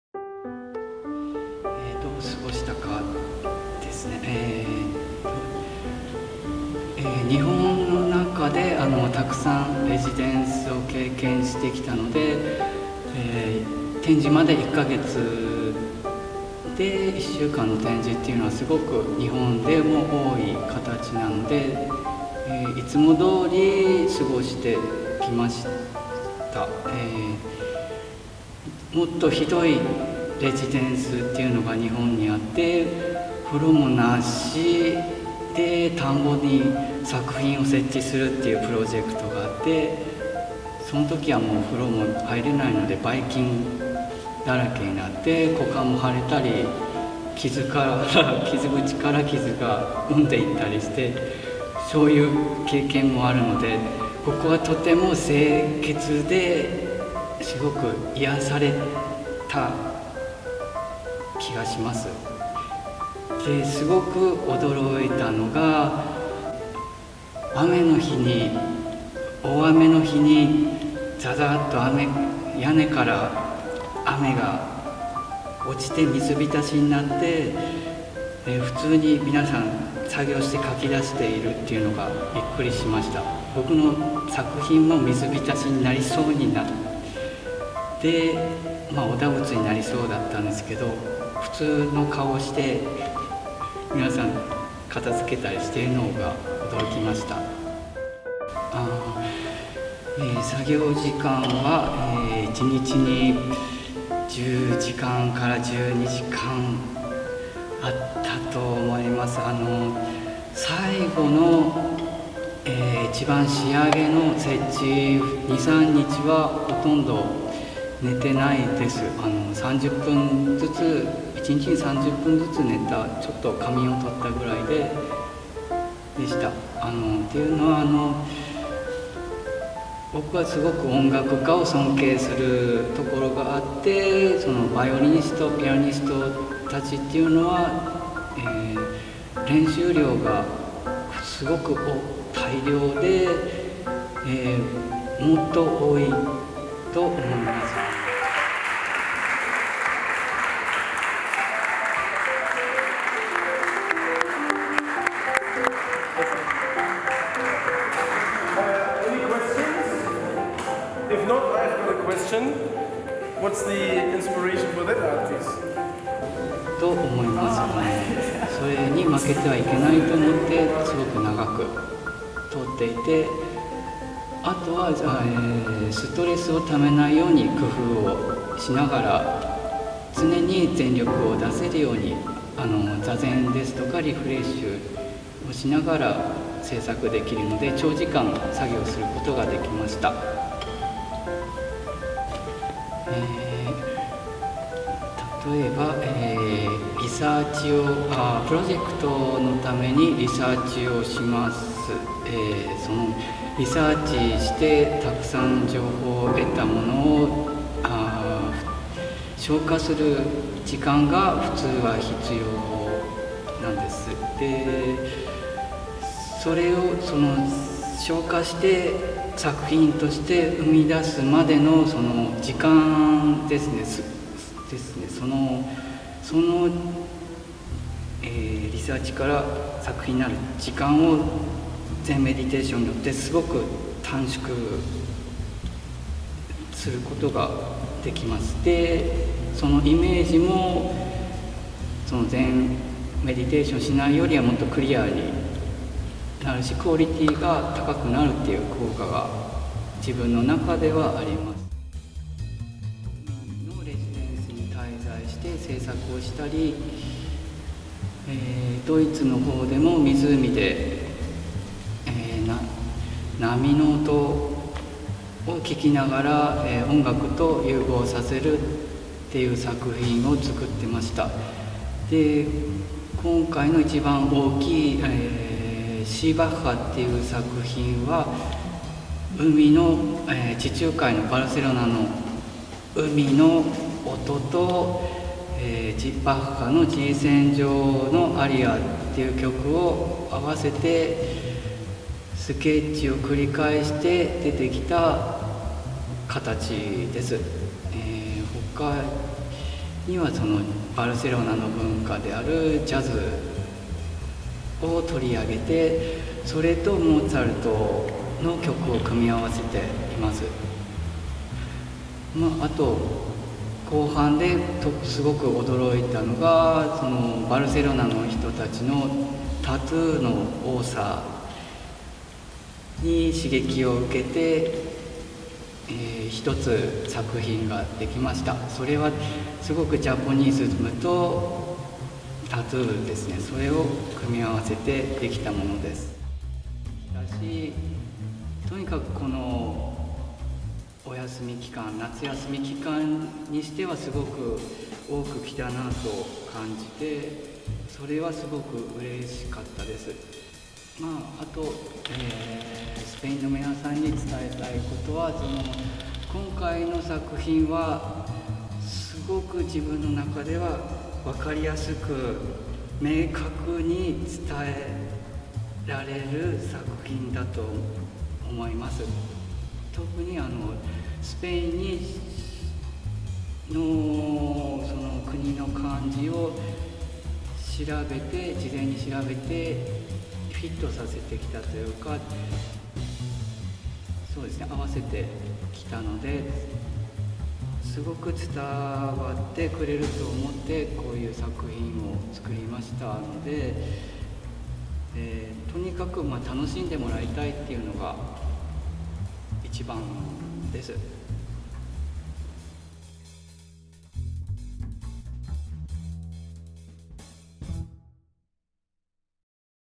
Kaoru Shibuta Intervieux by Espronceda
Winner of the Espronceda Open Call. Exhibition curated by Savina Tarsitano